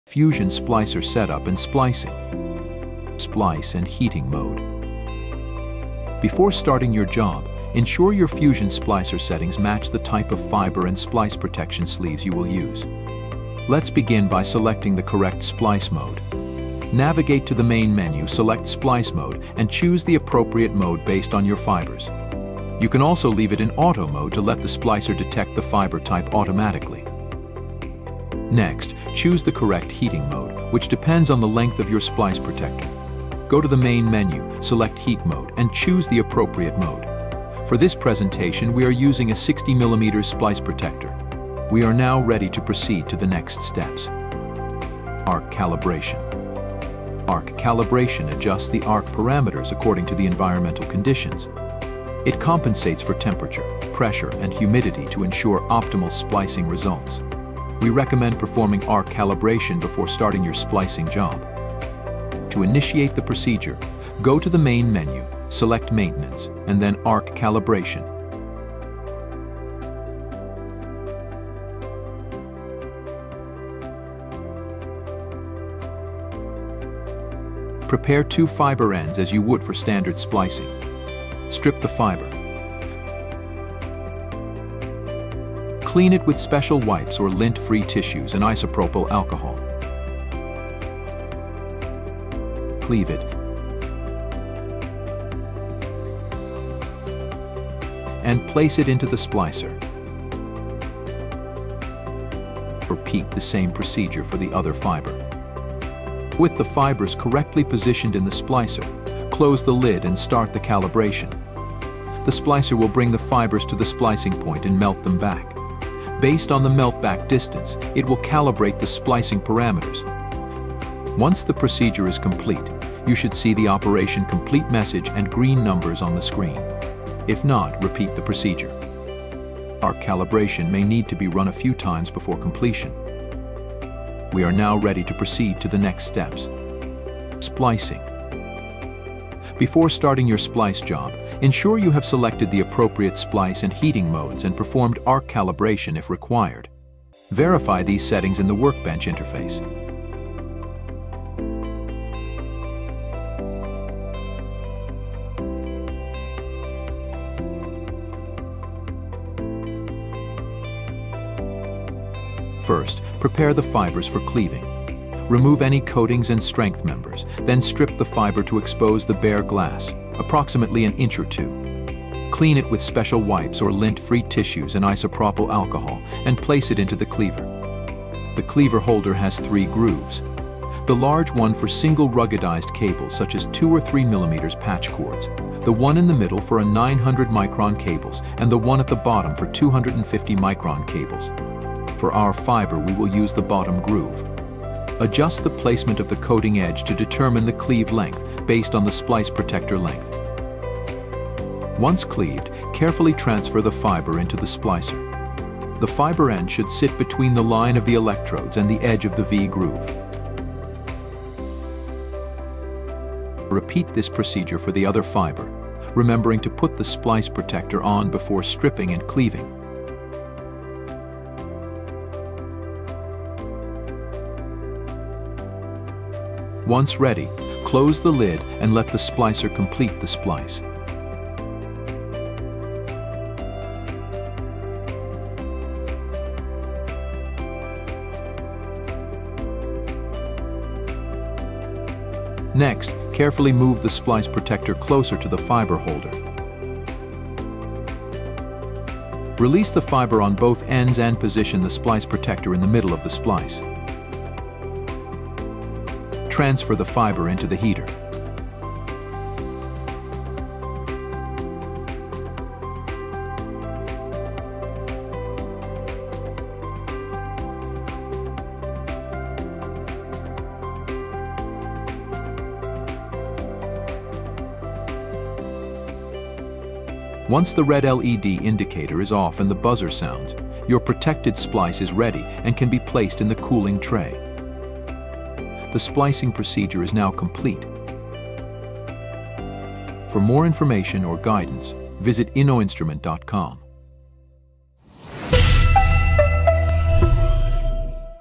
Fusion Splicer Setup & Splicing
In this video, you'll learn how to set up and use a fusion splicer for perfect splicing results. We cover essential steps including arc calibration, where you'll see a detailed, step-by-step walkthrough to set the correct arc power and duration for minimal splice loss and maximum efficiency.